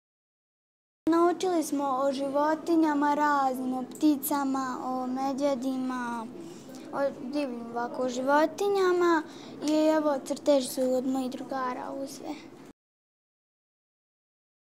KidsFestival Berina
Budi saradnik Nature: radionica Natura 2000 na Kids Festivalu Sarajevo 2014